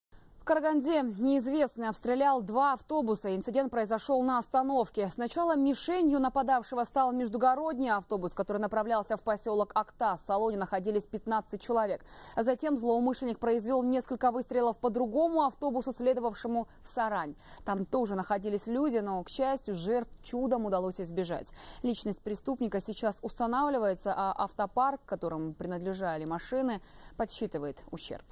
Неизвестный обстрелял два пассажирских автобуса в Караганде
Сначала мишенью нападавшего стал междугородний автобус, который направлялся в поселок Актас.